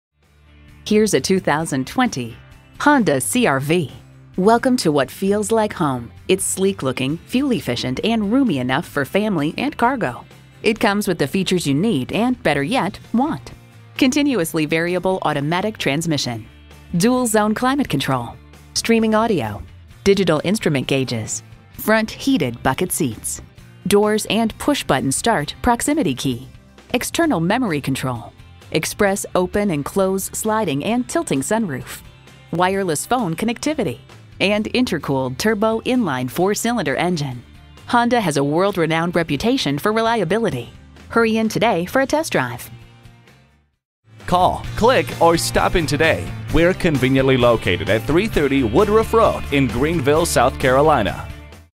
Used 2020 Honda CR-V Greenville SC Easley, SC SB025065B - SOLD
Year: 2020
Make: Honda
Model: CR-V
Trim: EX
Engine: 1.5L I4 DOHC 16V
Transmission: CVT
Color: Silver
Interior: Gray
Mileage: 37869
Stock #: SB025065B
VIN: 5J6RW1H54LA019543
Quite possibly the perfect car for you! Includes the FAMILY PLAN!!

Address:
330 Woodruff Road